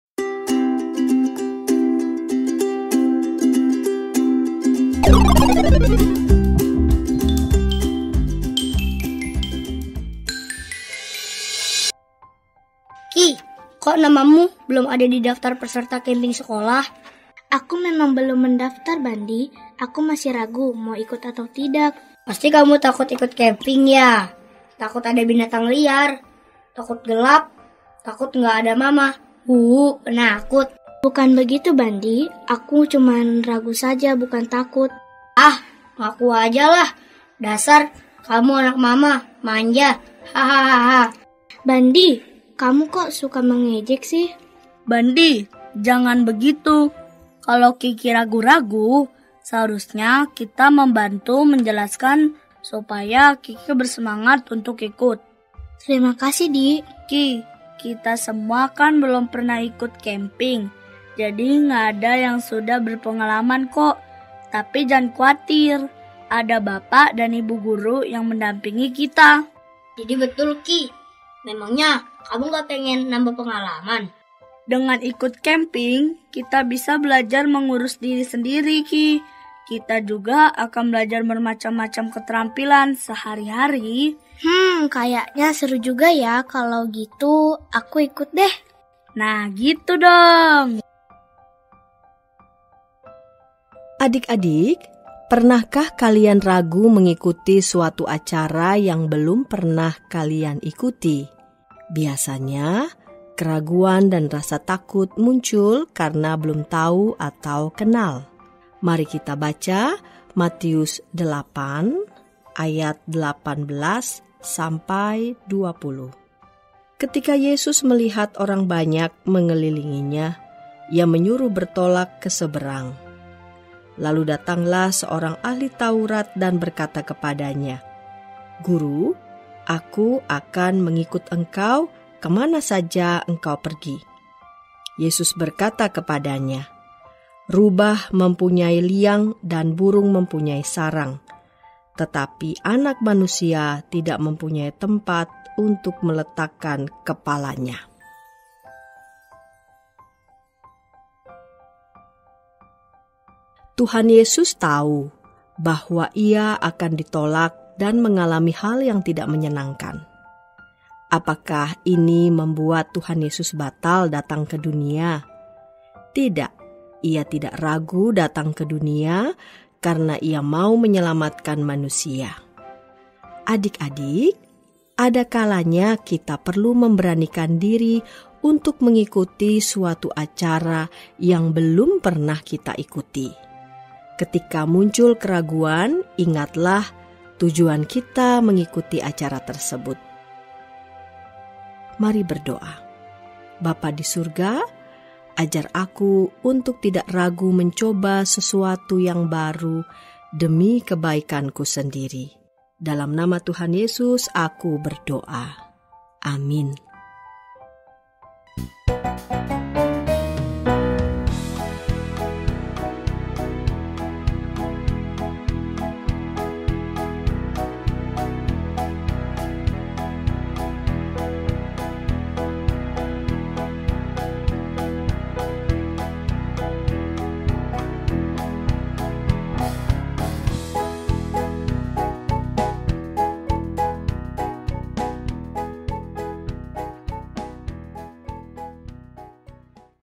KIDDY 989 - KIKI RAGU IKUT KEMPING
Renungan Harian Anak “Kiddy” untuk hari Jumat, 1 Maret 2024
Berjudul : KIKI RAGU IKUT KEMPING
Didasarkan pada : Matius 8:20